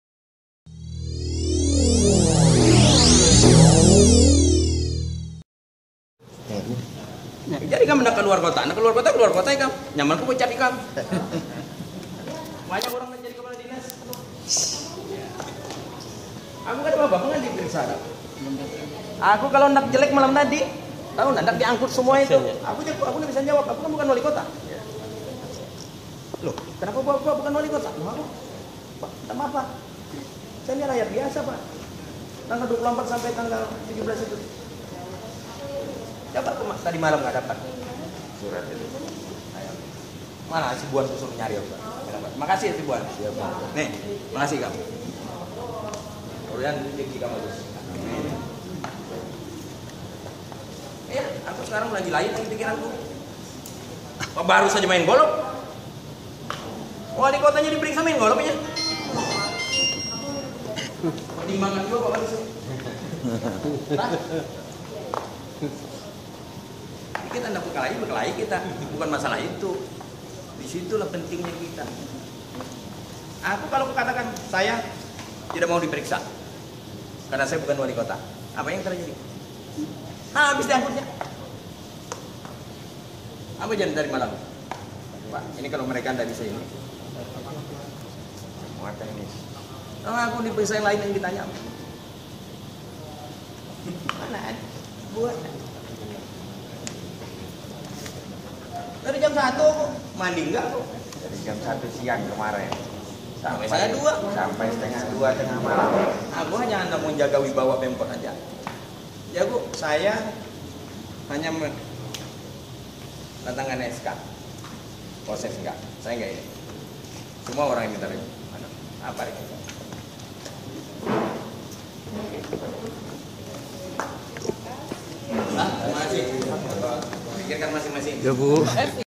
VIDEO Rekaman Walikota Samarinda Damprat Sejumlah Kepala Dinas Terkait OTT Pelabuhan
TRIBUNKALTIM.CO, SAMARINDA – Usai mengelar konfrensi pers, Walikota Samarinda Syahrie Jaang meluapkan kemarahan kepada beberapa Kepala Dinas, Minggu (19/3/2017), di aula rumah jabatan, Samarinda
Kemarahan Jaang dipicu ketidakpedulian kepala dinas terkait, saat Jaang diperiksa oleh Bareskrim Mabes Polri, Sabtu (18/3/2017), di Mako Brimob Samarinda Seberang.
Diketahui, nama Walikota Samarinda Syaharie Jaang ikut terseret dalam Operasi Tangkap Tangan (OTT) Mabes Polri dan Polda Kaltim, Jumat lalu.
Jaang terseret lantaran menandatangani Surat Keputusan (SK) Walikota tentang parkir di Terminal Peti Kemas (PTK) Palaran.